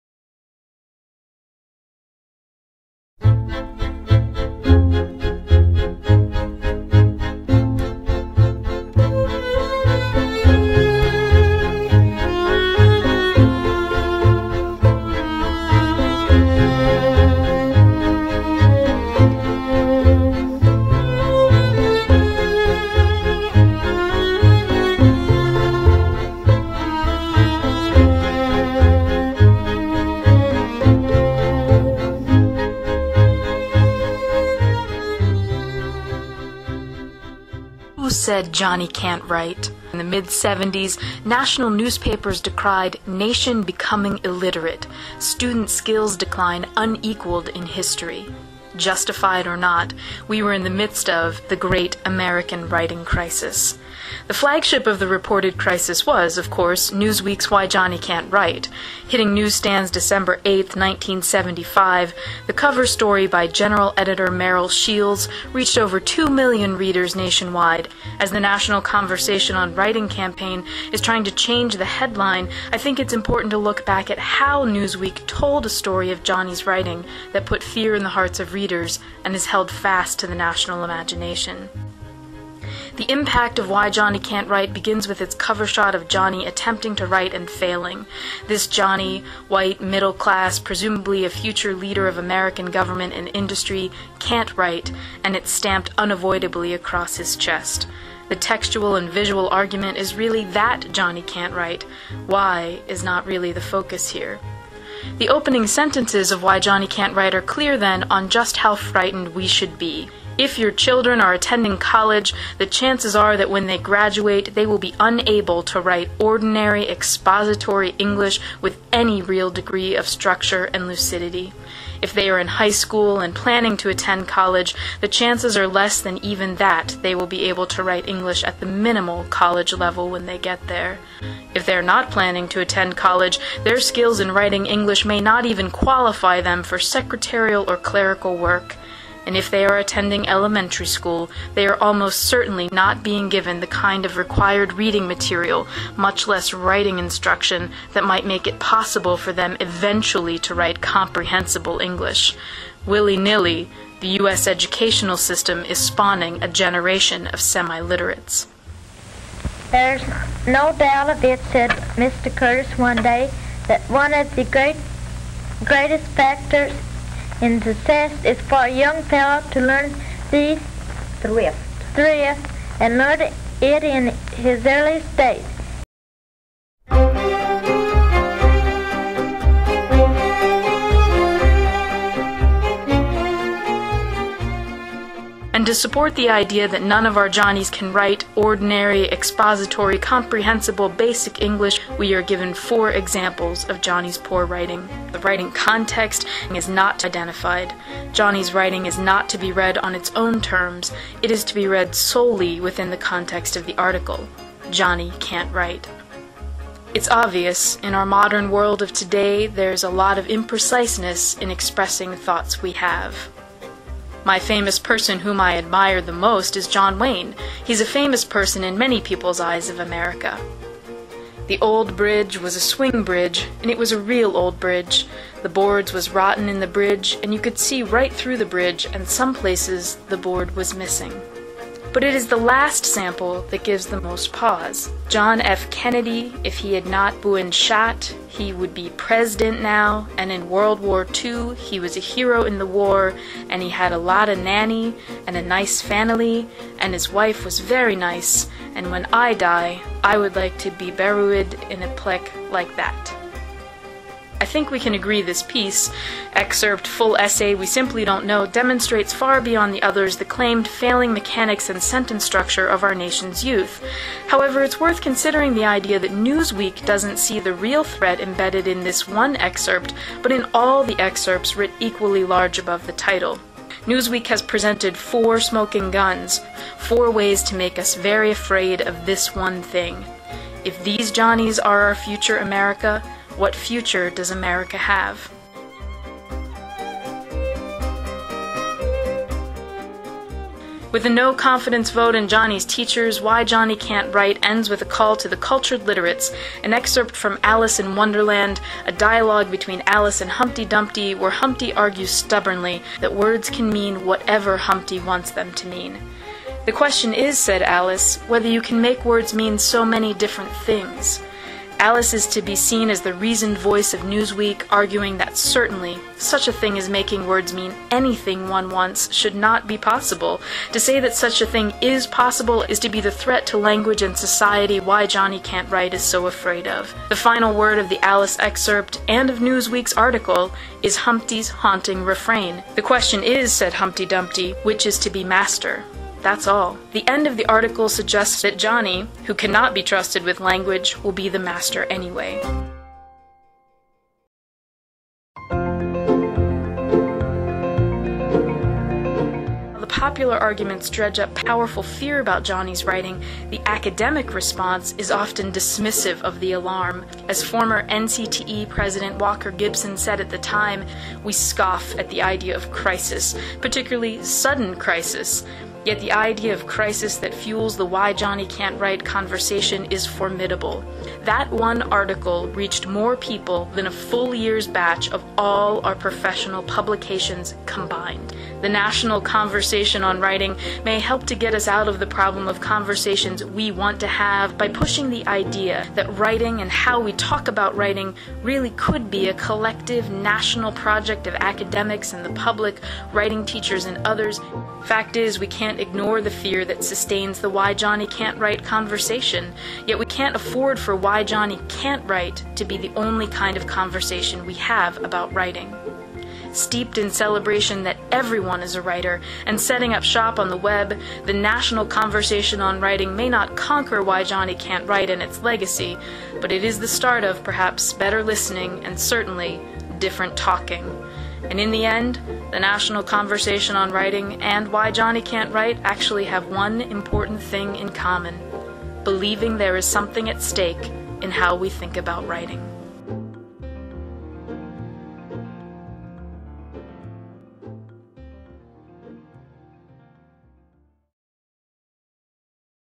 Why Johnny Can't Write
A video review of the famous Newsweek article 30 years later.